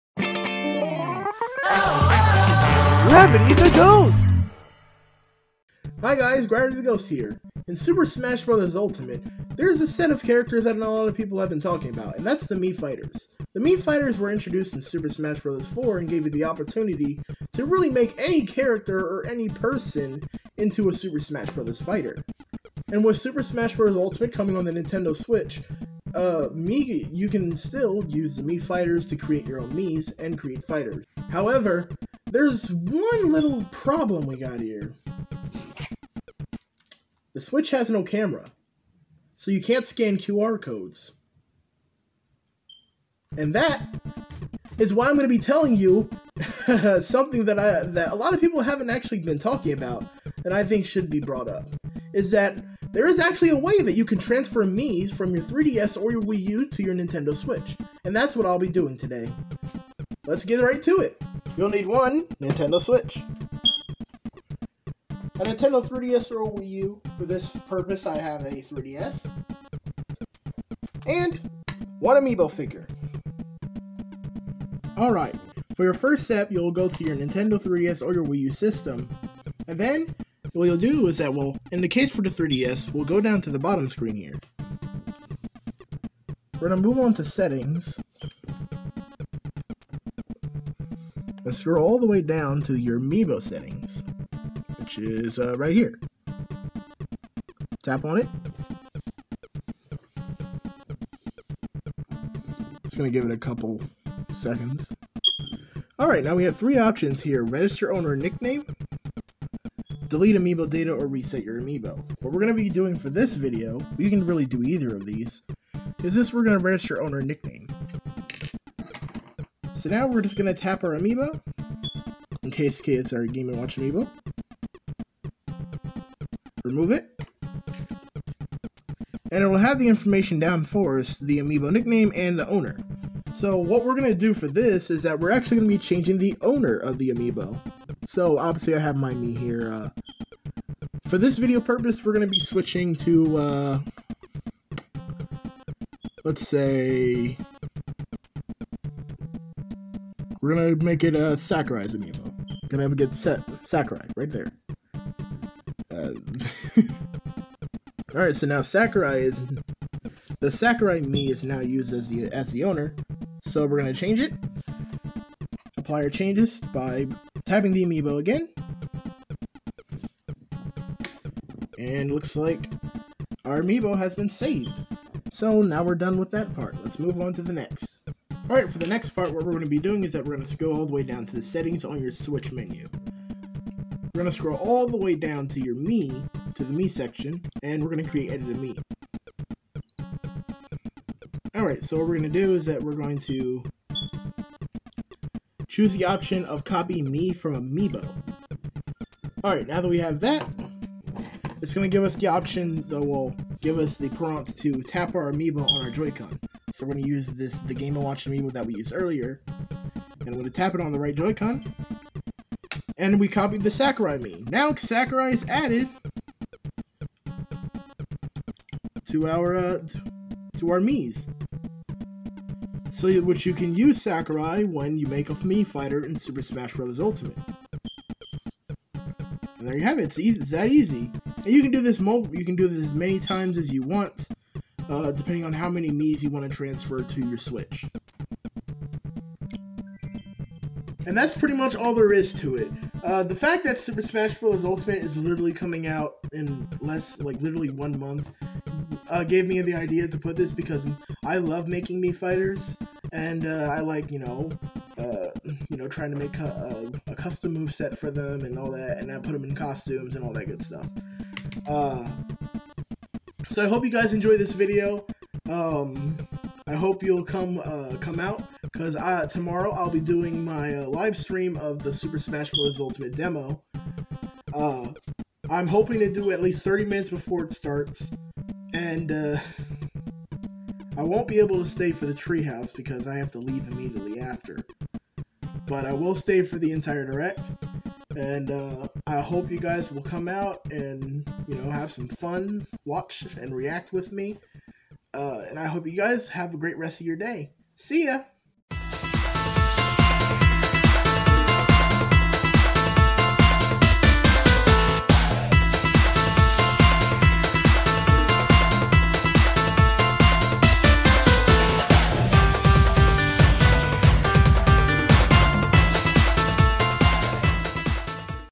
How to Transfer Miis From Your 3DS or Wii U to Nintendo Switch!
Mii Fighters have returned for Super Smash Bros. Ultimate! But...those QR codes for Miis can't be scanned on a Nintendo Switch. Here's a way you can transfer them in under five minutes!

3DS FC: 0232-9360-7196
Pokemon Showdown Name: GravityTheGhost